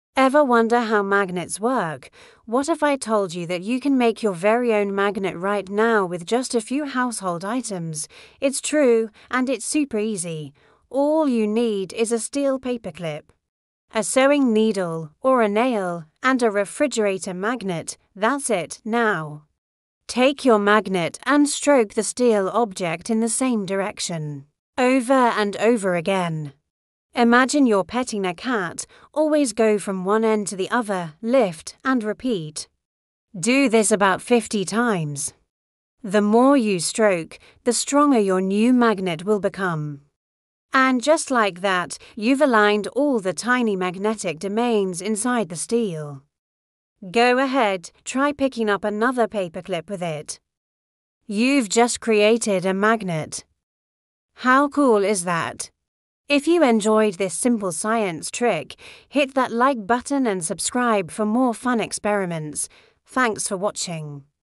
"جرب بنفسك: مغناطيس من مسمار وبطارية فقط!""DIY Magnet with Just a Nail and Battery!"
في هذا الفيديو، نأخذك في تجربة علمية بسيطة وممتعة: كيف تصنع مغناطيسًا باستخدام مسمار وبطارية فقط! تجربة مثالية للأطفال والكبار، تجمع بين التعلم والمتعة. لا تنسَ الإعجاب، والاشتراك، ومشاركة الفيديو مع أصدقائك لتعم الفائدة!
In this video, we show you how to make a simple magnet at home using just a nail and a battery! A fun and educational DIY experiment for kids and adults alike.